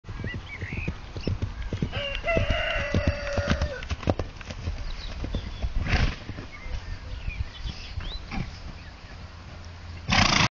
Lottie
Unfortunately my phone battery died in the middle of filming, so you only get a very shoty video! Lottie, showing that Shire horses can move after all. :)